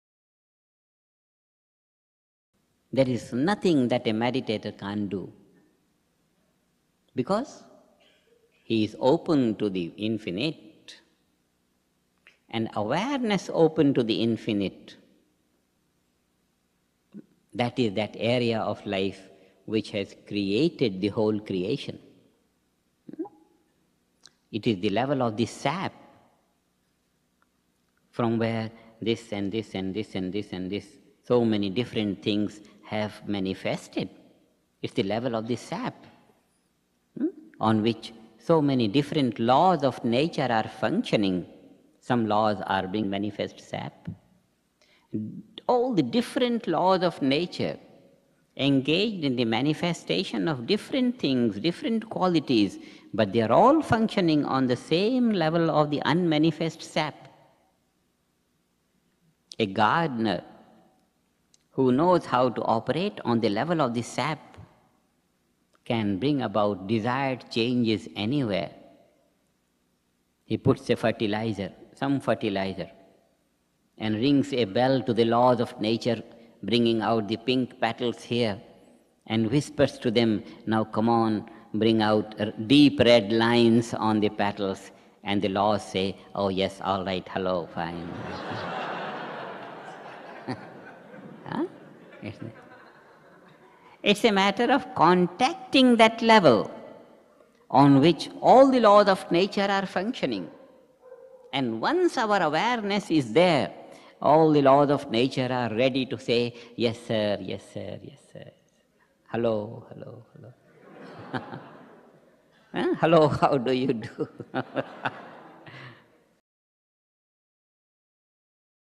Maharishi Mahesh Yogi on the TM meditator
"There is nothing that a meditator can’t do because he is open to the infinite…” — Maharishi Mahesh Yogi, founder of the TM technique